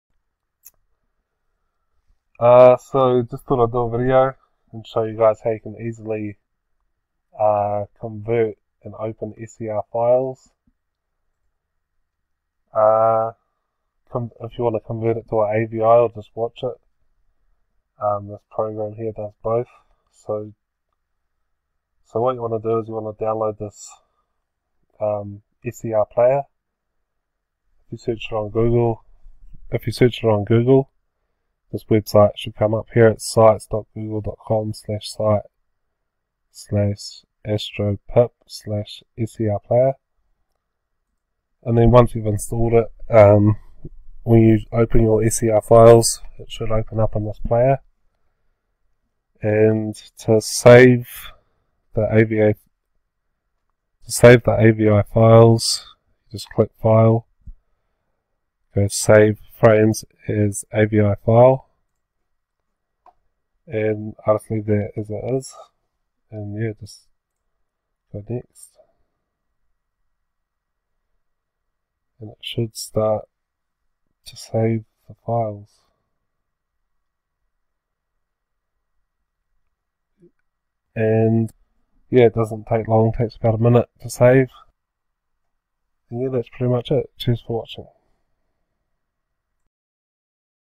How To Open And Watch SER Files And Convert To AVI
Sorry audio not so good don't think I had my microphone plugged in properly but you should still learn how to convert ser files to avi files or just how to view them as videos.